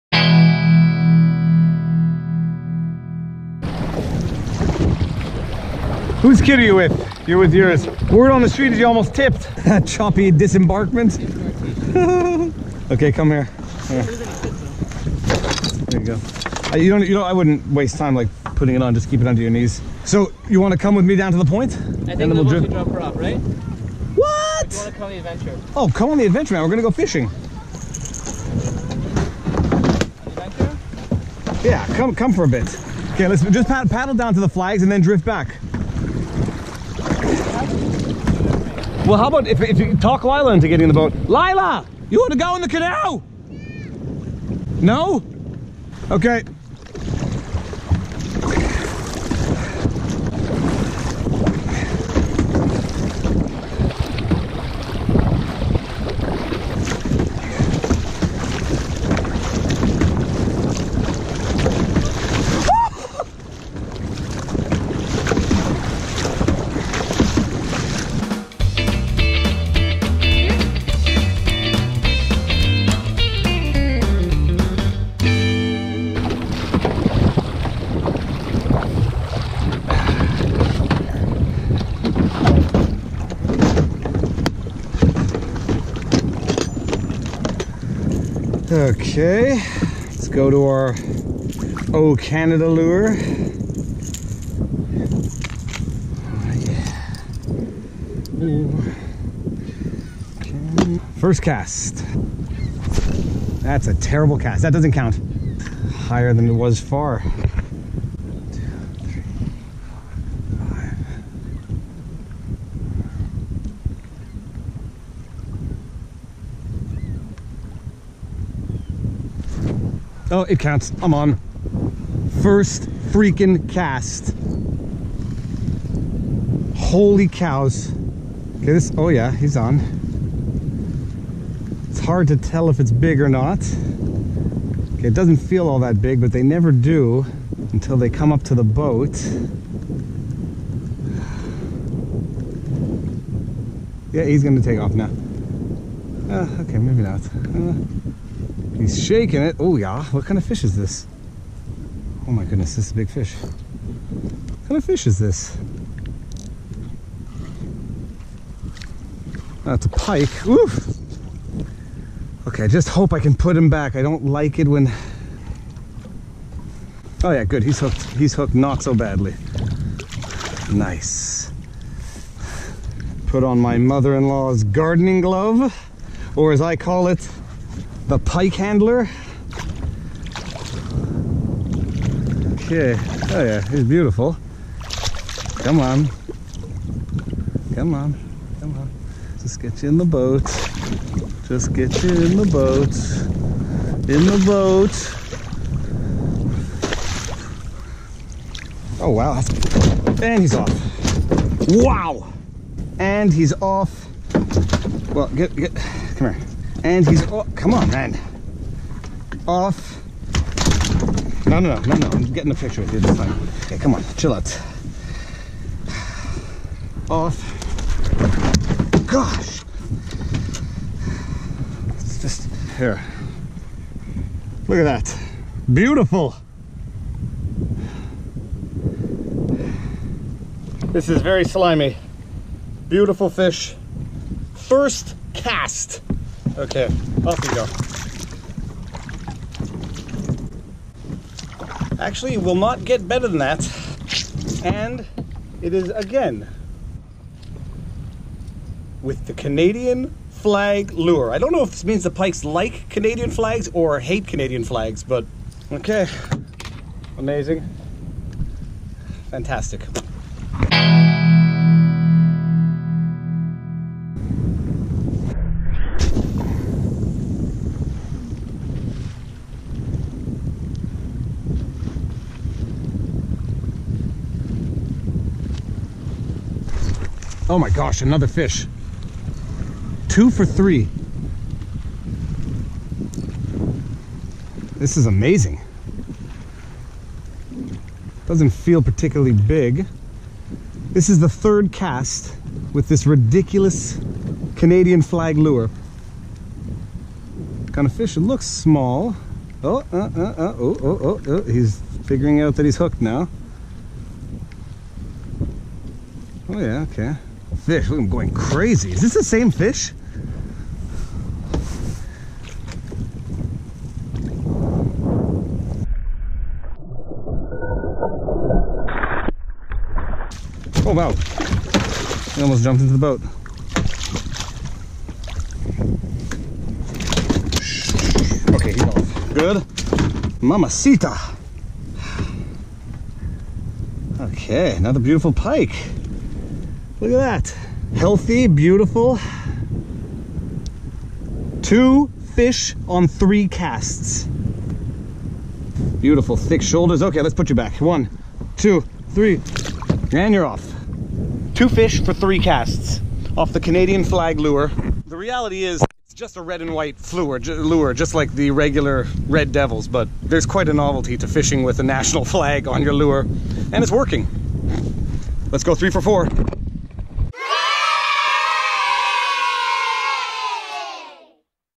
ANOTHER MONSTER PIKE ON MY FIRST CAST!!!
Another weekend, another awesome experience. I literally too 5 casts, then retired for the day. On the first cast, I caught a magnificent specimens of a pike. And again on the 3rd cast. Both were released unharmed. After the 5th cast, I decided not to push my luck and went back for a day with the kids. Peace out!